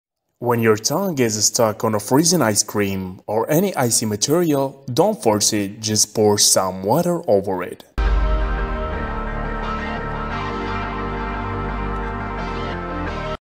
Young girl got her tongue stuck on a freezing ice cream🍦😂
when your tongue is stuck on a freezing ice cream, or any icy material, don't force it, just poor some water over it